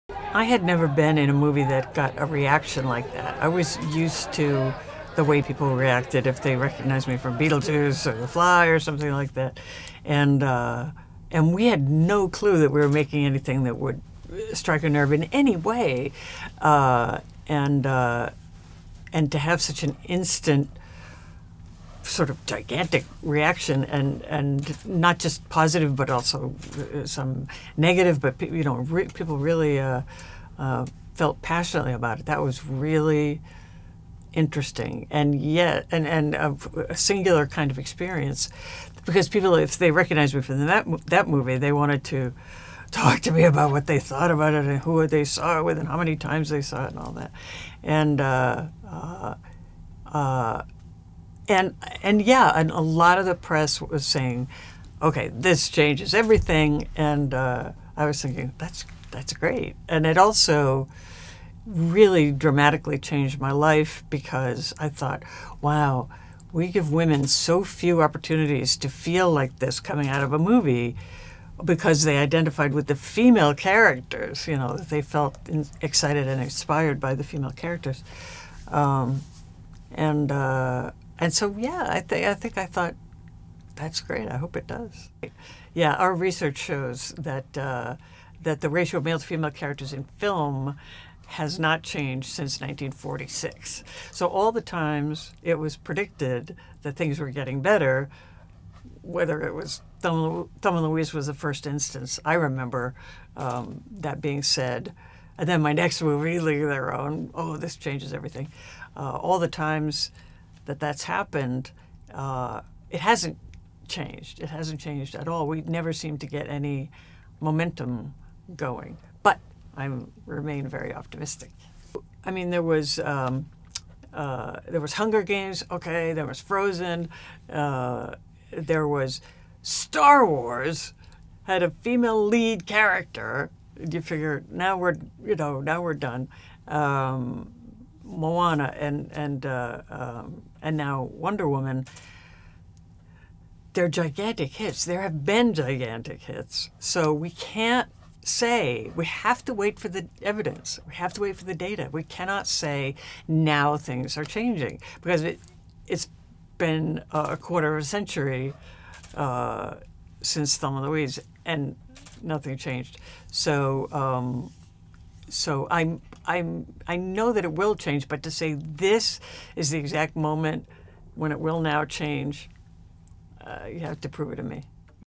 Geena Davis: 'Thelma &amp; Louise' didn't propel female led-films
More than a quarter-century after ‘Thelma &amp; Louise’ became a hit, the film remains an anomaly rather than a ground-breaker in terms of women’s roles in Hollywood. A group of studies _ including one from the institute started by one of the movie’s stars, Geena Davis, to expose gender bias _ show that movies are still dominated by men, onscreen and behind the camera. (Aug. 30)